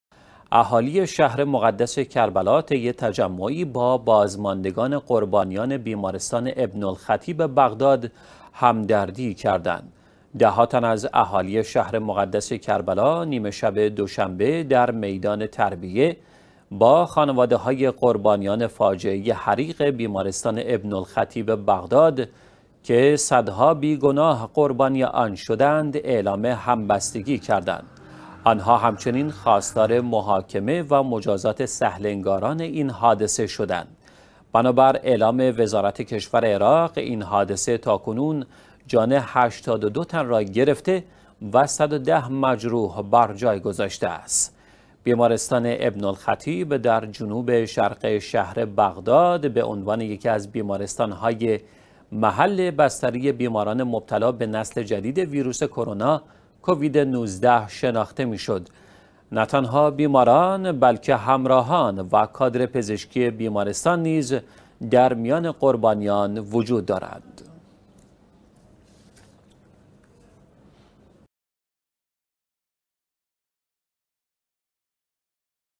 تظاهرات مردم شهر مقدس کربلا درهمبستگی با قربانیان ابن الخطیب
اهالی شهر مقدس کربلا طی تجمعی با بازماندگان قربانیان بیمارستان ابن الخطیب بغداد همدردی کردند.
ده ها تن از اهالی شهر مقدس کربلا، نیمه شب دوشنبه، در میدان تربیه، با خانواده های قربانیان فاجعه حریق بیمارستان ابن الخطیب بغداد که صدها بی گناه قربانی آن شدند، اعلام همبستگی کردند.
آنها همچنین خواستار محاکمه و مجازات سهل انگاران این حادثه شدند.
بنابر اعلام وزارت کشور عراق، این حادثه تاکنون جان 82 تن را گرفته و 110 مجروح بر جای گذاشته است.
بیمارستان «ابن الخطیب» در جنوب شرق شهر بغداد به عنوان یکی از بیمارستان های محل بستری بیماران مبتلا به نسل جدید ویروس کرونا (کووید-۱۹) شناخته می شد.
نه تنها بیماران بلکه همراهان و کادر پزشکی بیمارستان نیز در میان قربانیان وجود دارند.